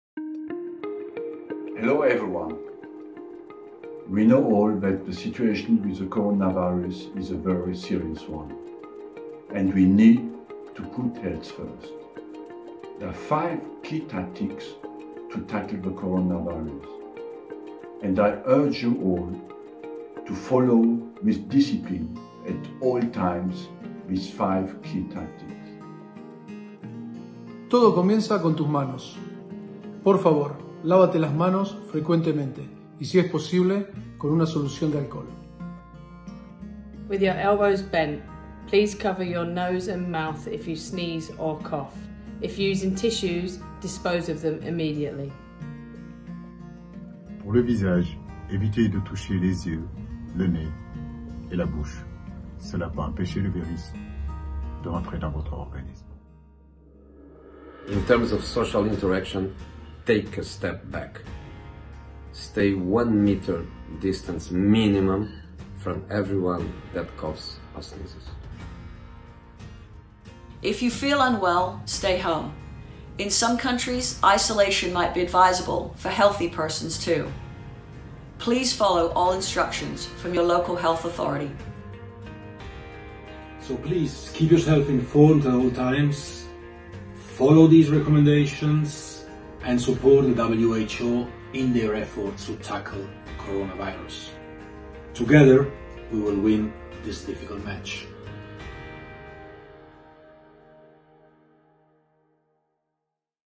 FIFA and WHO launch five key tactics to tackle coronavirus
FIFA has reiterated its support to the World Health Organization (WHO) in fighting the coronavirus (COVID-19) by launching an awareness campaign led by six world-renowned coaches, who request everyone across the world to follow five key tactics that tackle the spread of the disease.

With tactics playing such a key role in football success, the coaches are united behind the five-step game plan to defeat the virus – hands, elbow, face, distance and feel – in line with the WHO’s basic protective measures against COVID-19.

Be READY to fight COVID19. For all the latest health advice, go